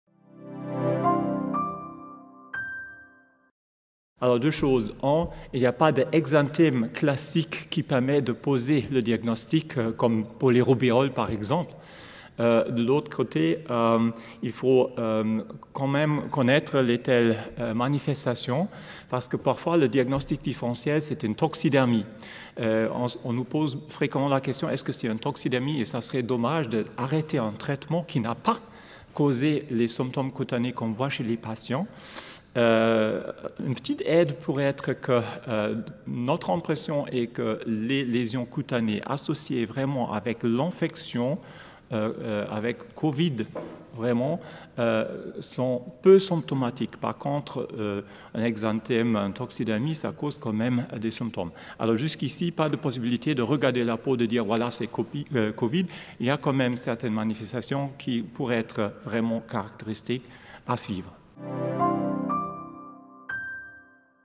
Certaines atteintes cutanées peuvent-elles être un des symptômes de la maladie Covid-19 ?
Certaines atteintes cutanées peuvent-elles être un des symptômes de la maladie Covid-19 ? La réponse du Pr Wolf-Henning Boehncke du Service de dermatologie et vénéréologie des HUG, dans le cadre des colloques du mardi de médecine des HUG, le 14 avril 2020 : Point de situation à propos du coronavirus de la maladie COVID-19.